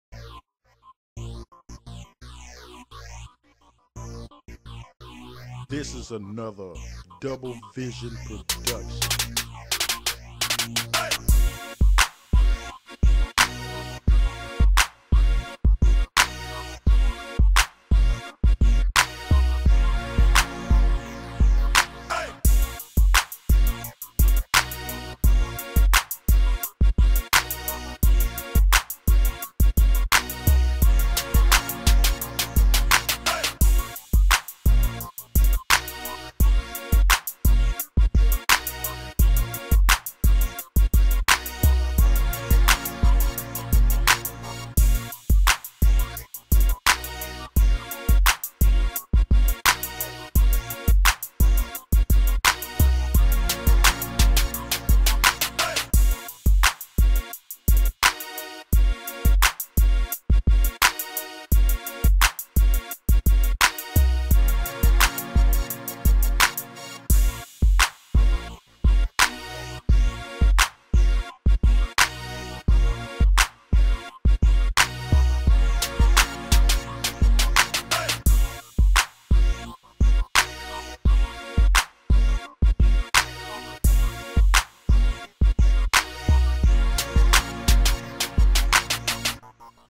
BEATS ON DECK! (Doublevision Productions)
This is another Dirty South type banger from Twinnsoniq of Doublevision Productions. More beats available for lease or purchase @ I GOT EM' 4 CHEAP... Get at me!